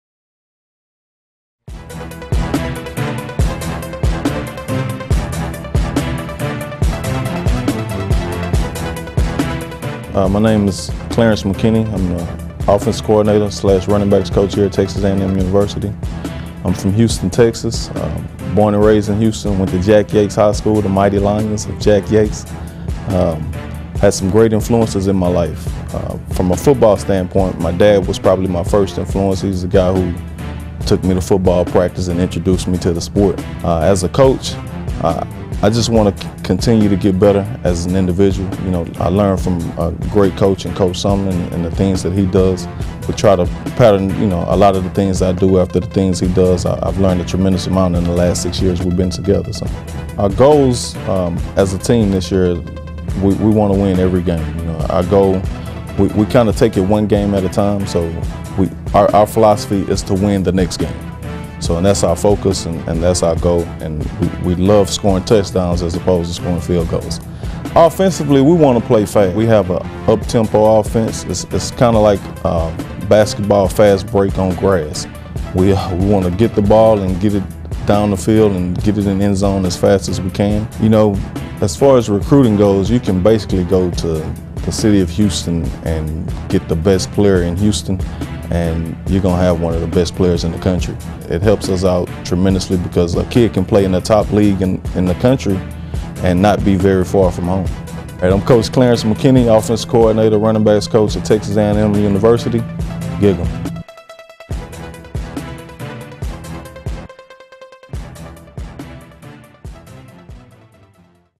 Hey Coach: Clarence McKinney
Meet coach Clarence McKinney, Houston native and our OC/Running Backs coach.